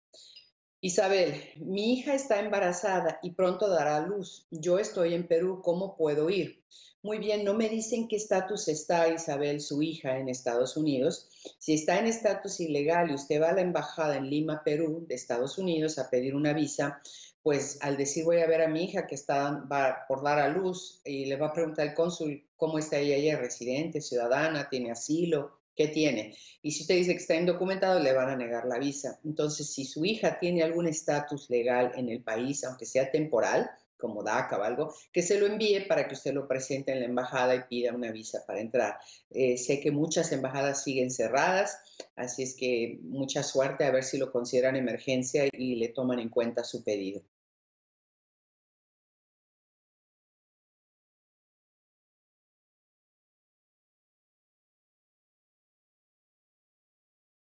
¿Cómo puedo ir a Estados Unidos para visitar a mi hija a punto de dar a luz? | Noticias Telemundo
Video oficial de Noticias Telemundo. Depende mucho del estatus migratorio de su hija, si ella está indocumentada, hay muchas posibilidades que el cónsul le niegue la visa a Ud.